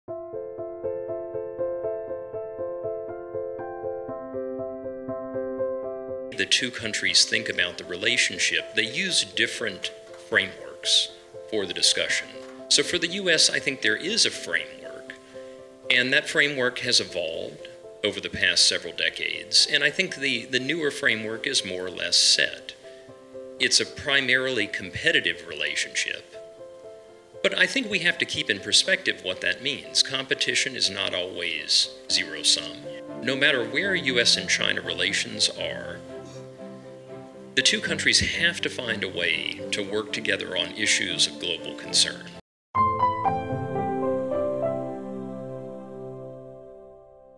美国前官员华自强：无论美中关系如何发展 两国都须找到合作之道
第十一届北京香山论坛于9月12日至14日在北京国际会议中心举行。9月12日，在“高端访谈·地区安全：中美关系”环节中，欧亚集团中国部门常务董事、美国前助理国务卿帮办华自强表示，美中两国在描述双边关系时用的是不同的框架，过去几年间美国对华关系的框架在不断变化。在他看来，当前美方新的框架主要是竞争关系，但要认识到这种竞争并不总是零和博弈。无论美中关系如何发展，两国都须找到一种在全球关注问题上的合作之道。